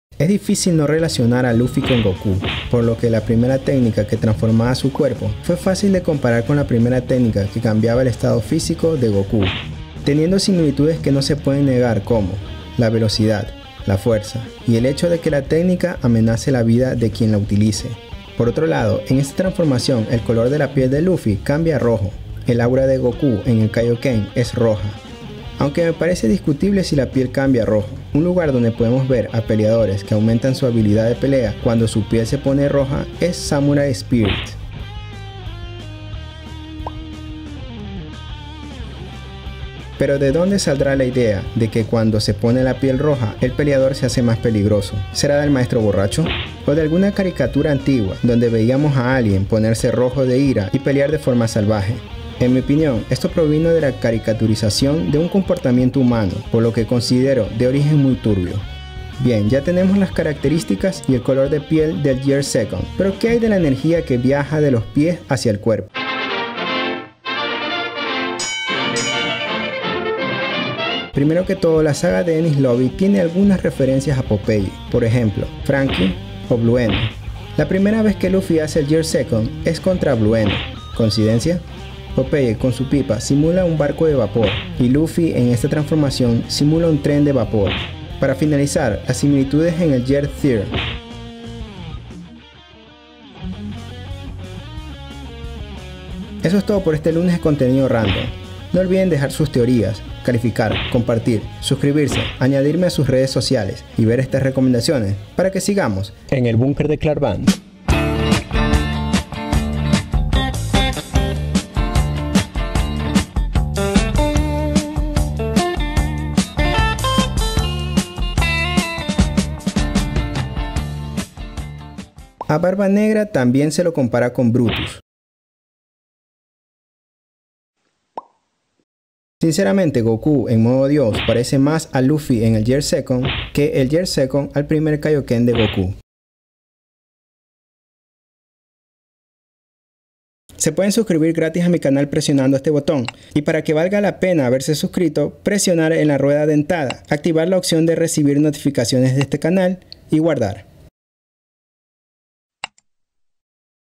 ONE PIECE: EL GEAR SECOND ESTÁ BASADO EN POPEYE? Curiosidades by ClarkKane
Popeye el marino sirvió de inspiración para la saga de Enies Lobby y el Gia Sekando de Monkey D. Luffy.